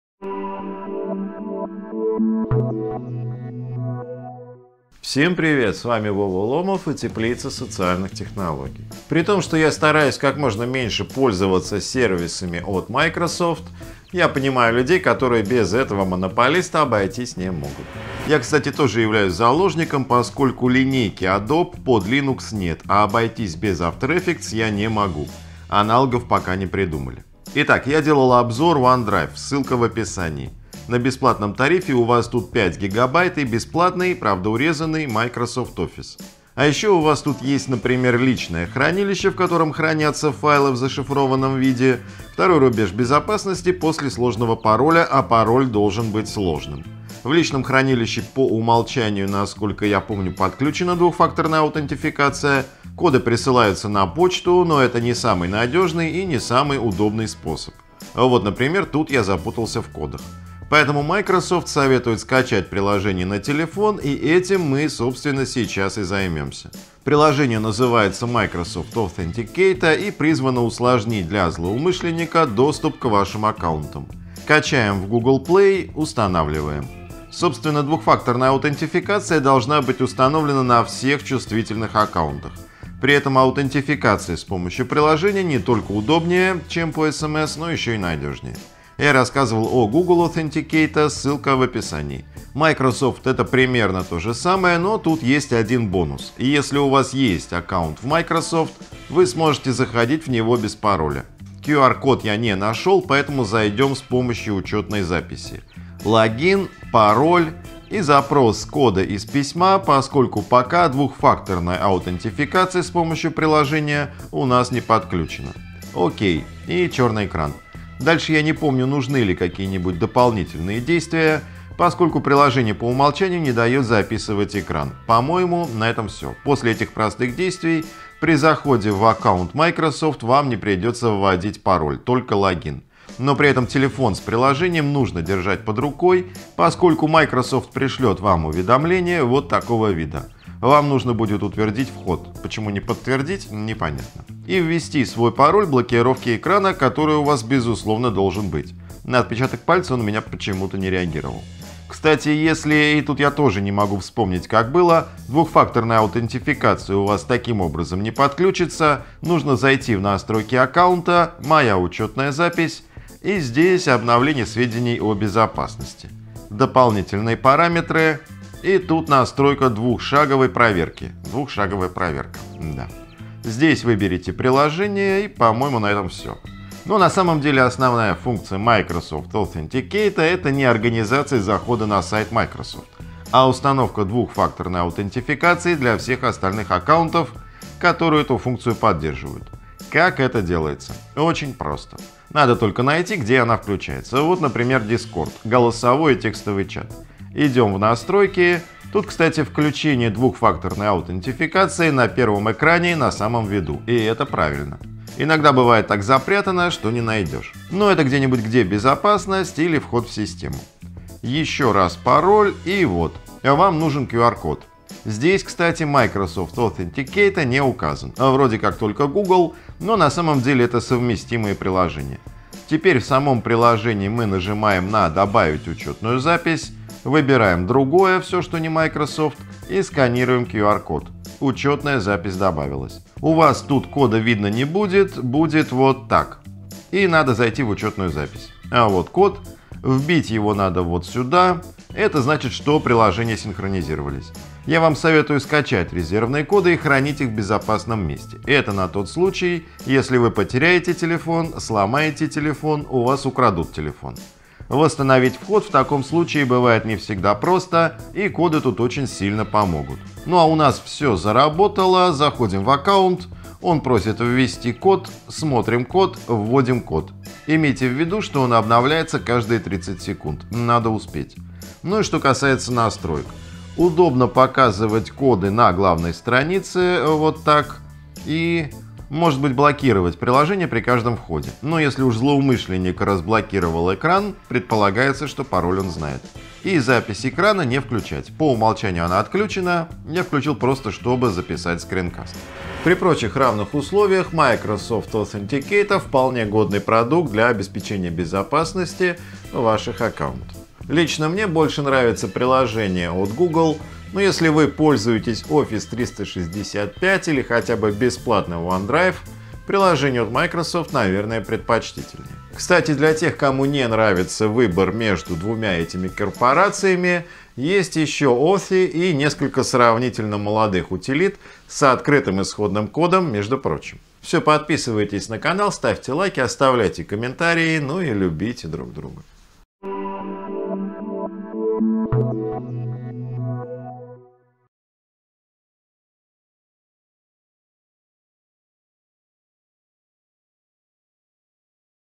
Microsoft Authenticator: как работает приложение для двухфакторной аутентификации
Microsoft Authenticator — приложение, призванное добавить безопасности вашим аккаунтам. То, что двухфакторная аутентификация должна быть подключена на всех ваших чувствительных аккаунтах — это общее место и про это не стоит напоминать.

Но и сам процесс двухфакторной аутентификации может осуществляться по-разному. Специалисты советуют использовать для этого именно коды из приложений для телефона, как наиболее безопасный способ. Таких приложений на сегодняшний день существует несколько: есть Authy Google Authenticator, о котором мы уже делали скринкаст и Microsoft Authenticator, о котором расскажем сегодня.